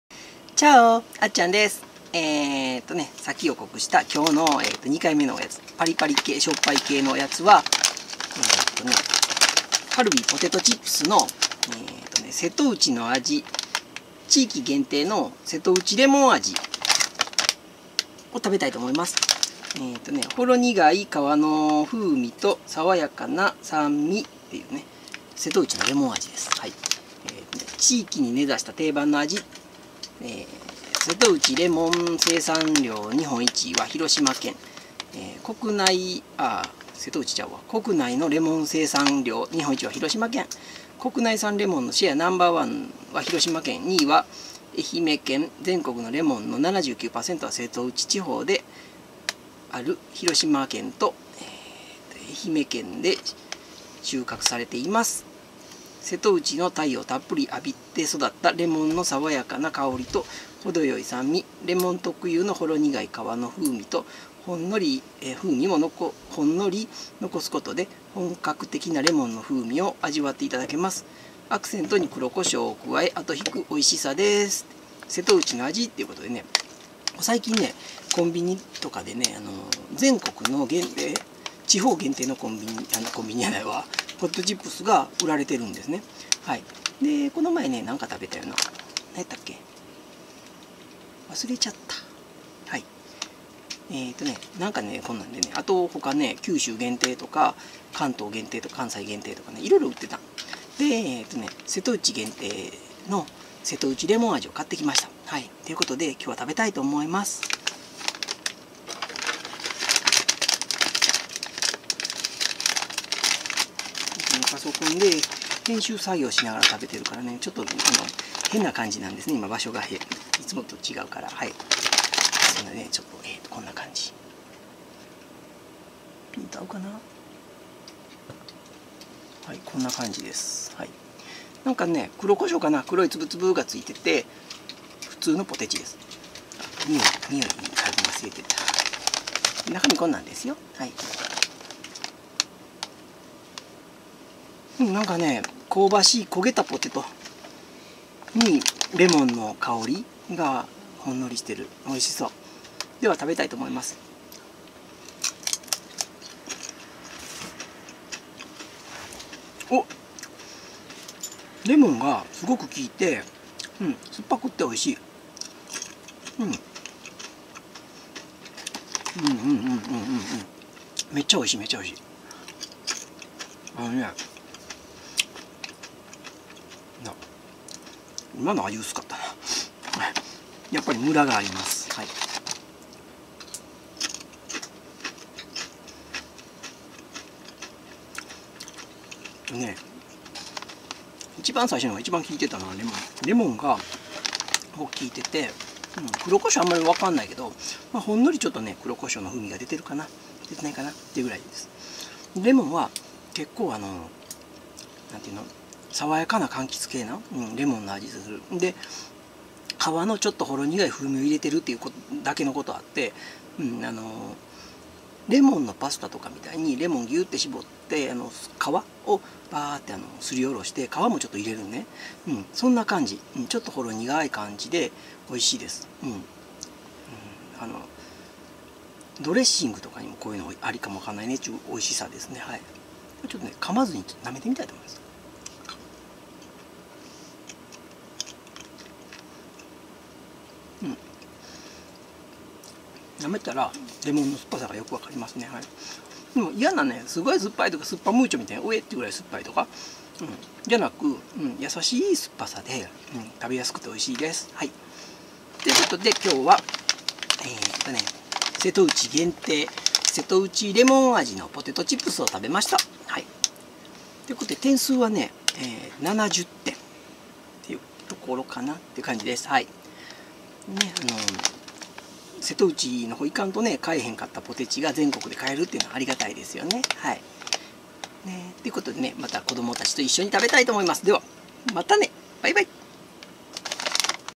地域限定ポテトチップス 瀬戸内レモン味 食べてみました☆
地域限定のポテチ　瀬戸内れモナ次食べて見ました
すっきりした味で、おいしかったよｖ